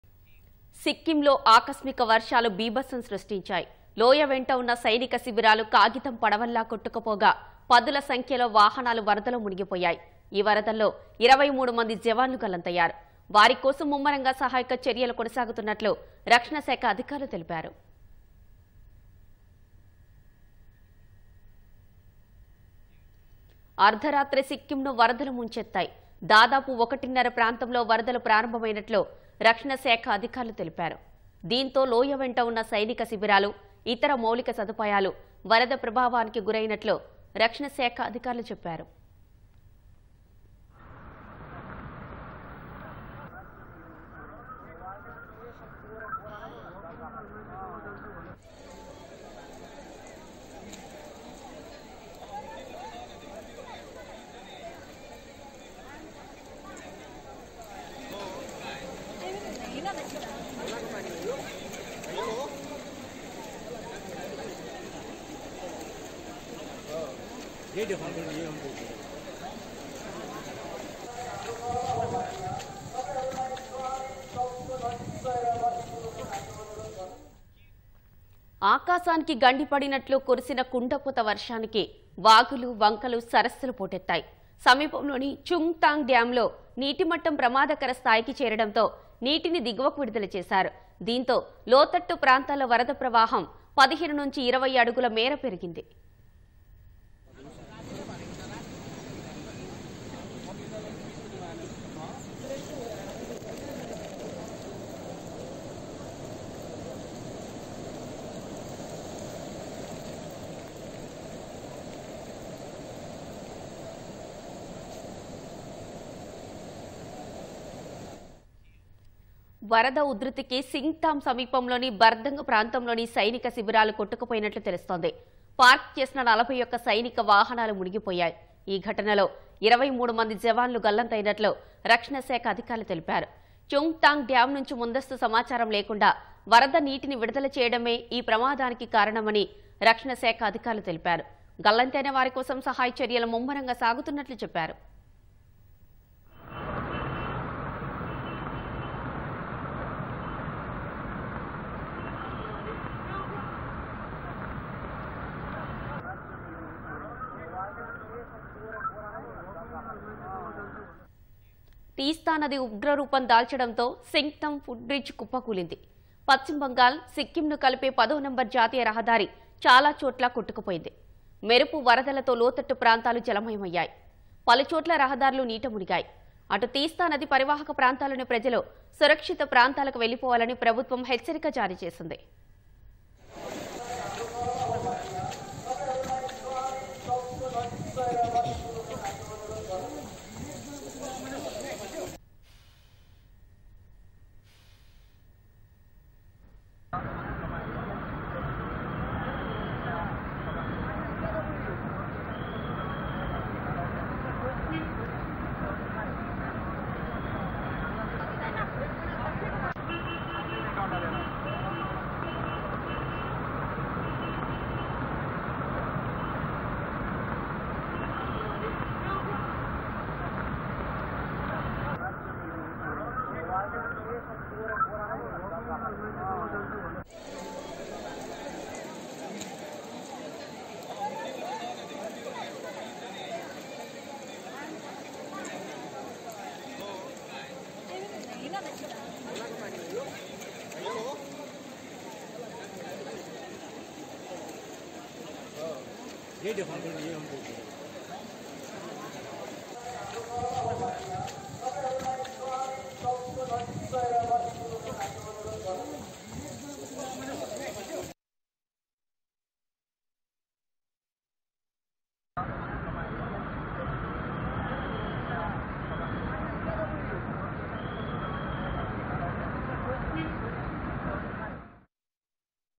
23 Army Personnel Missing After Floods Wreak Havoc In Sikkim | సిక్కింలో 23 మంది జవాన్లు గల్లంతు
సిక్కింలో ఆకస్మిక వర్షాలు బీభత్సం సృష్టించాయి. లోయ వెంట ఉన్న సైనిక శిబిరాలు కాగితం పడవల్లా కొట్టుకుపోగా. పదుల సంఖ్యలో వాహనాలు వరదలో మునిగిపోయాయి.
ఈ వరదల్లో 23 మంది జవాన్లు గల్లంతయ్యారు. వారికోసం ముమ్మరంగా సహాయక చర్యలు కొనసాగుతున్నట్లు. రక్షణ శాఖ అధికారులు తెలిపారు.